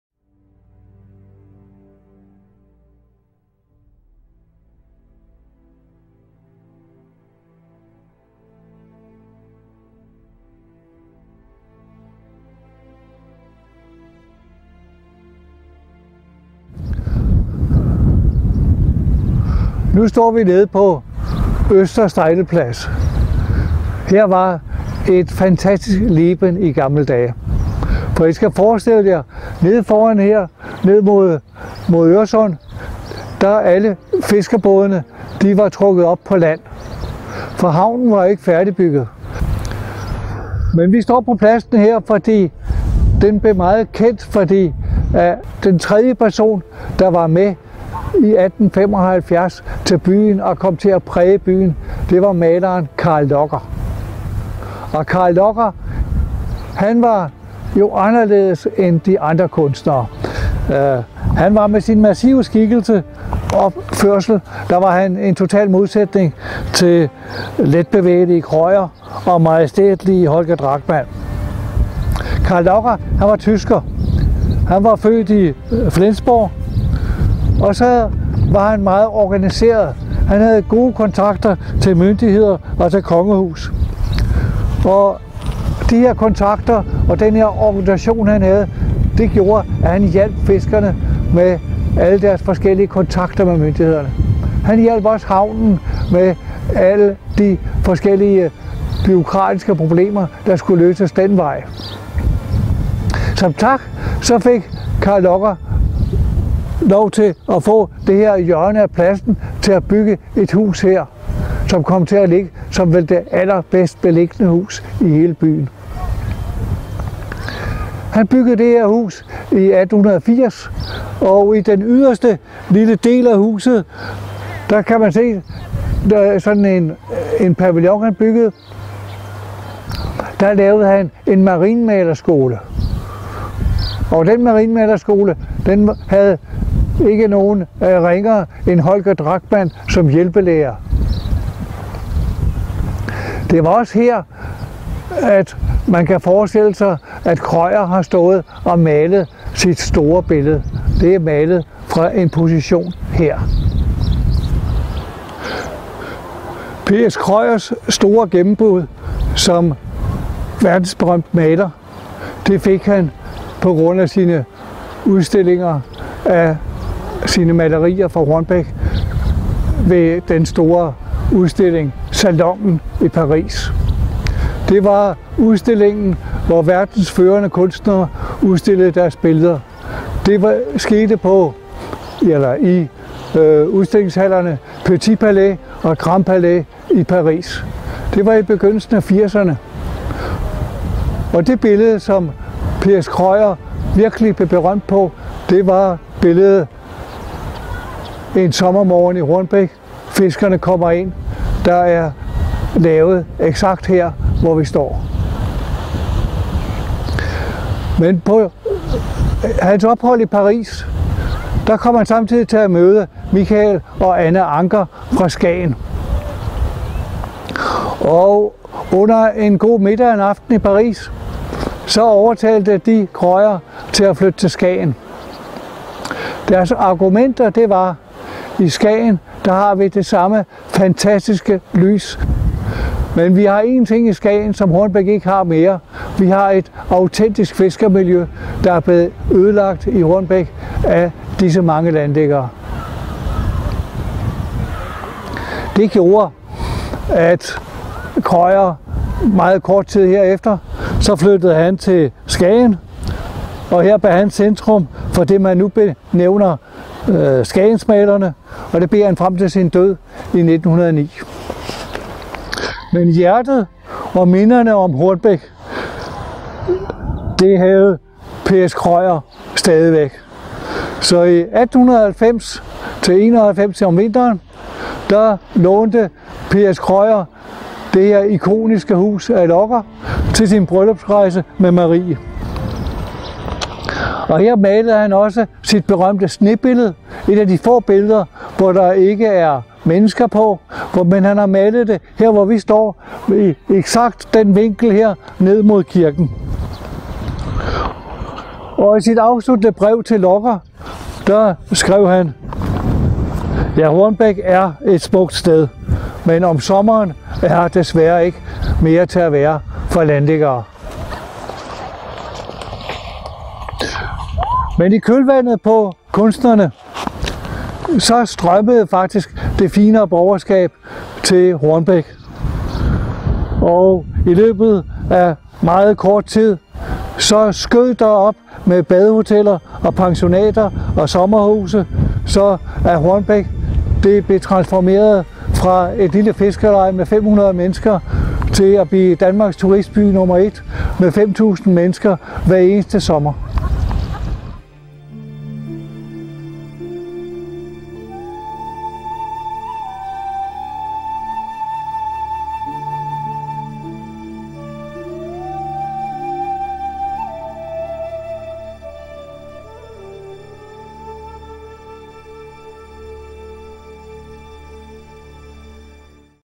Historien om P.S Krøyers og Carl Locher afsked med Hornbæk
P.S Krøyer blev forført til Skagen og Carl Locher forlod også Hornbæk. Se og hør William Leedgaards fortælling om de 2 kunstners afsked med Hornbæk.